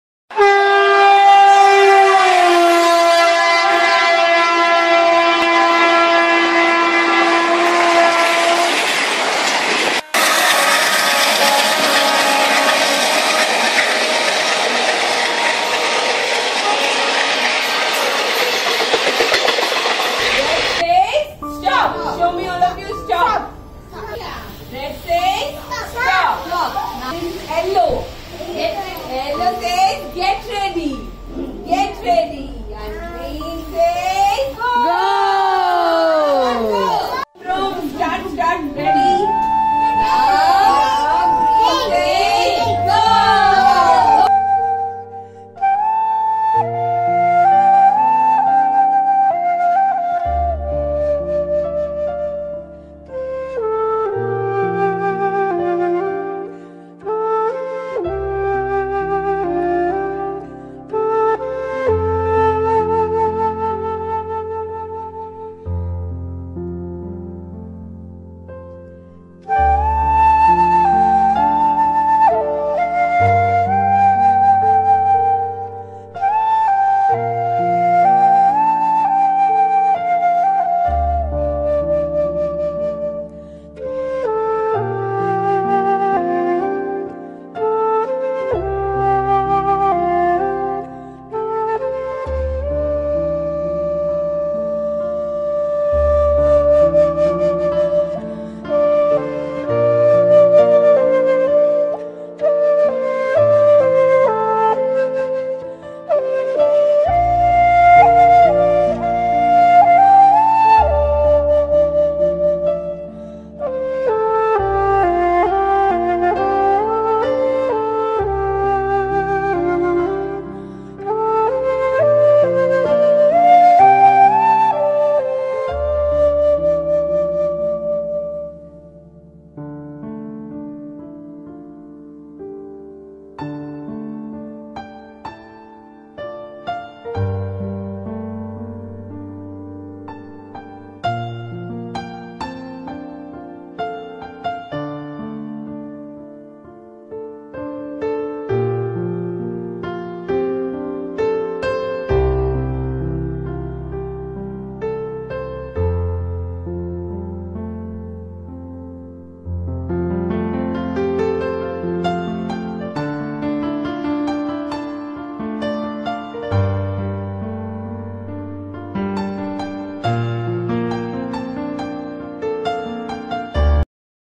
KIDS ACTIVITY VIDEO @ LITTLE SHISHYAS PRESCHOOL.
LITTLE SHISHYAS PRESCHOOL- THINKING BEYOND ACADEMICS.
ACTIVITY VIDEO:
TRAIN PLAY: Improves peer group interaction and team building.
WHEELBARROW WALK: Wheelbarrow walk. The wheelbarrow walk is a great upper extremity strengthening activity. You can make it easier or harder depending on where you put your hands that are holding their legs.
BUBBLE PLAY: A sensory and a calming down activity for kids.